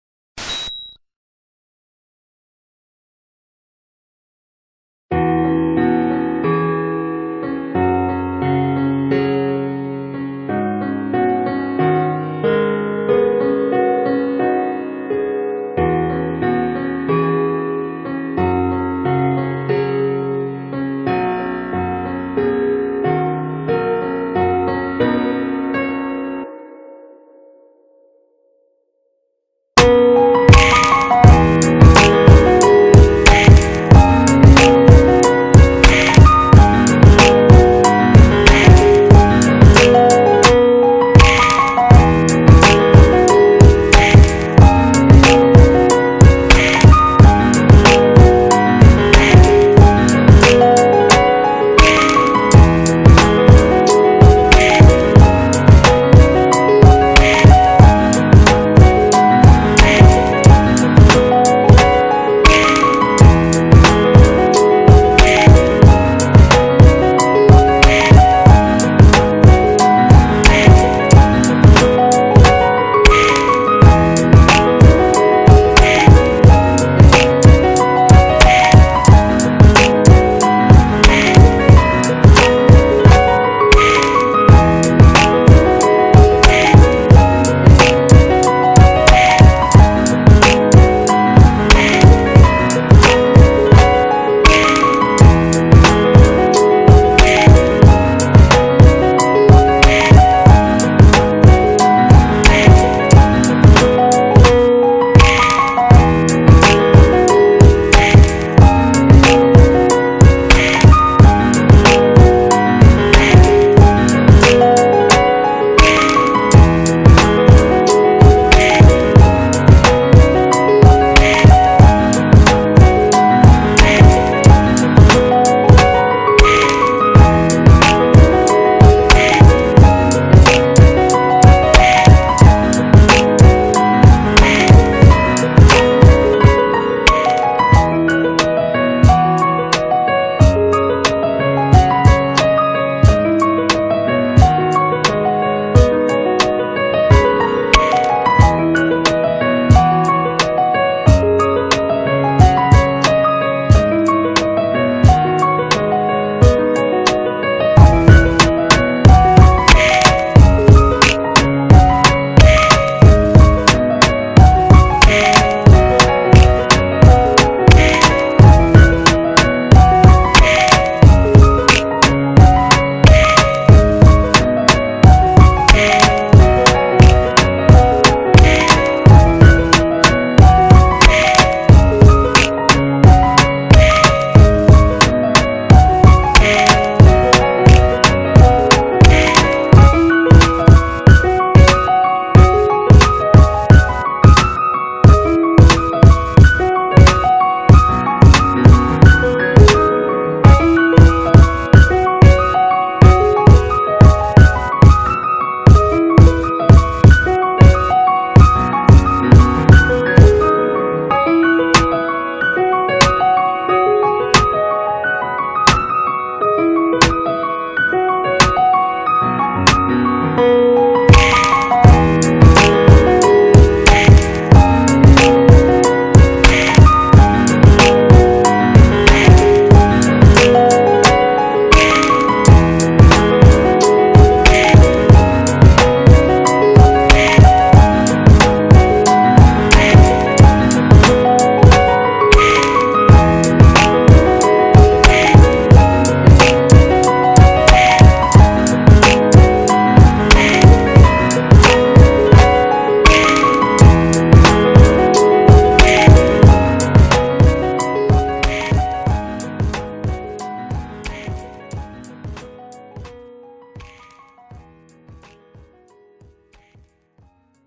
LUNCHCRUNCH | Pocahontas
Quick speed sculpt of Pocahontas. Heavily influenced by Glen Keane's design once again.